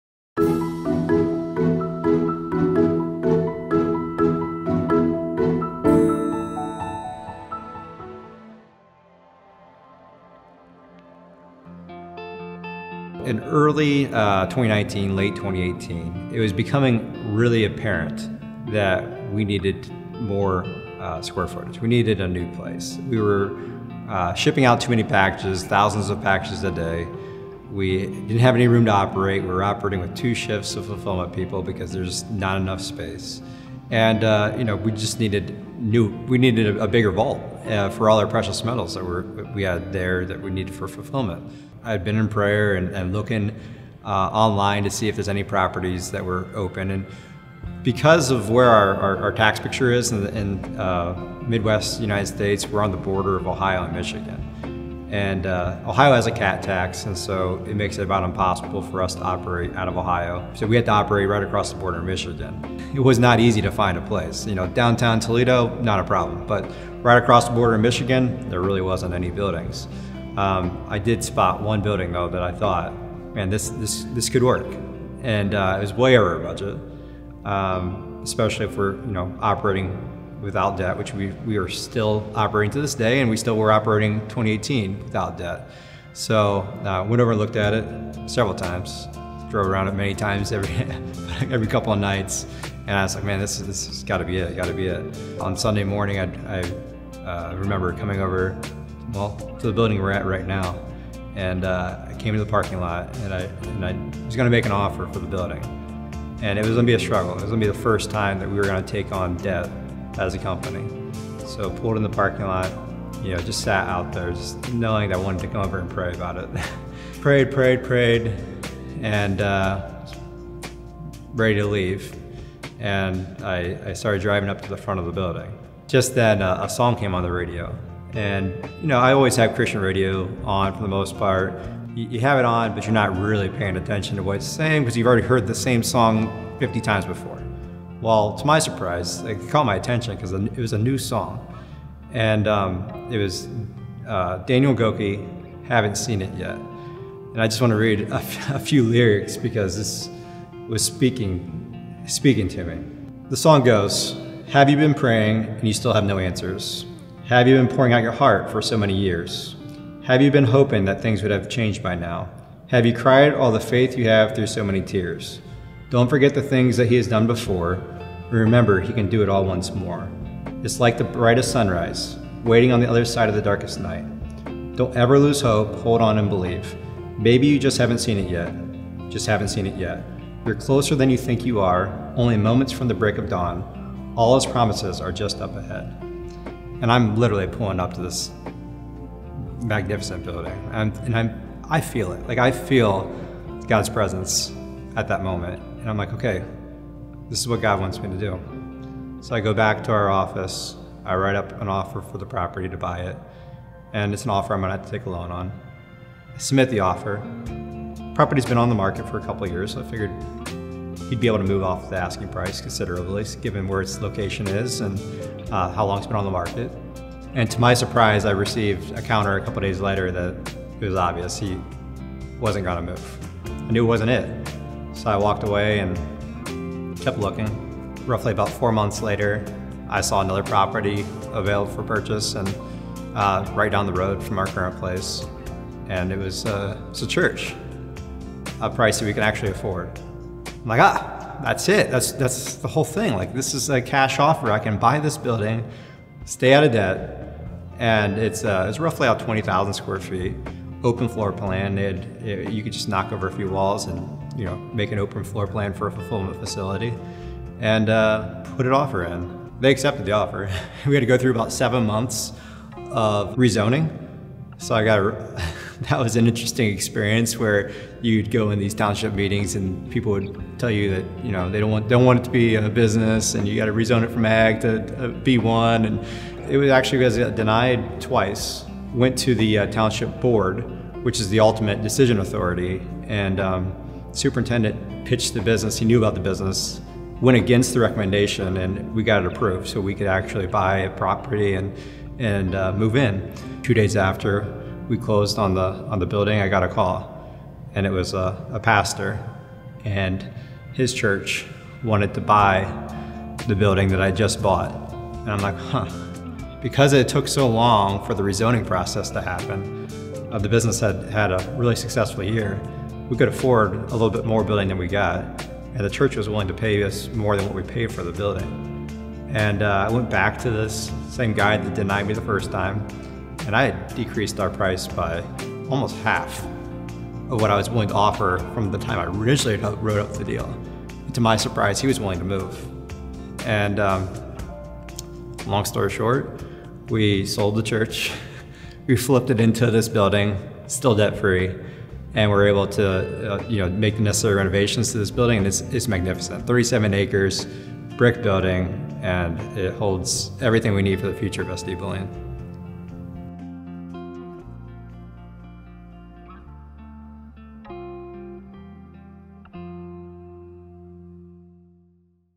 A Bigger Building - SD Early Years ep.7
In early 2019, it was becoming really apparent that we needed more space to operate our business of. Listen as I tell the incredible story of how God provided the perfect building for us. It's not what I thought it was going to be. He had something much bigger planned for us.